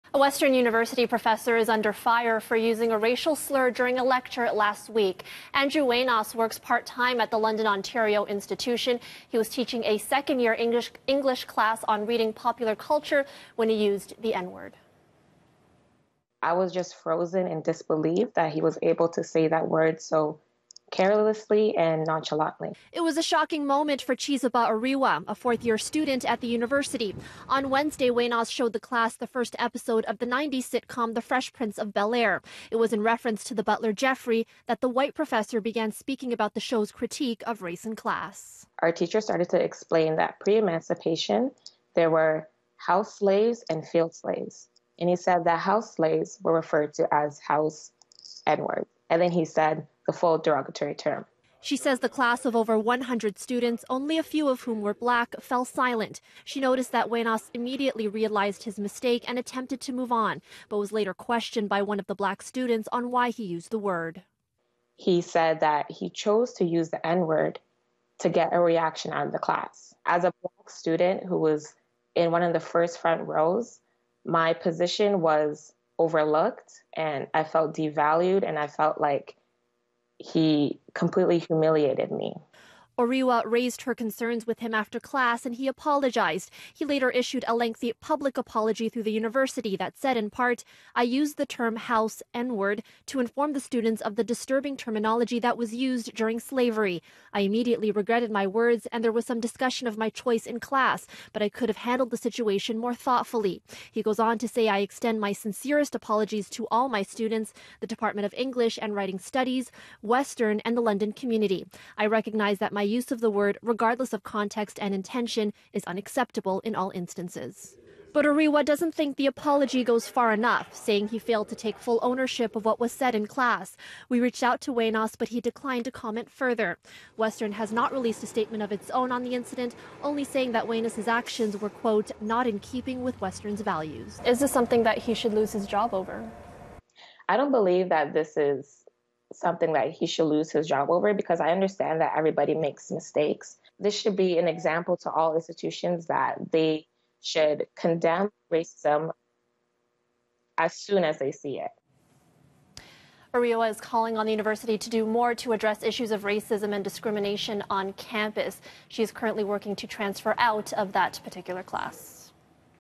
Western professor under fire for using N-word in lecture
An English lecturer at Western University has apologized for using the N-word during one of his classes. Erica Natividad spoke to one student who says his apology doesn't go far enough.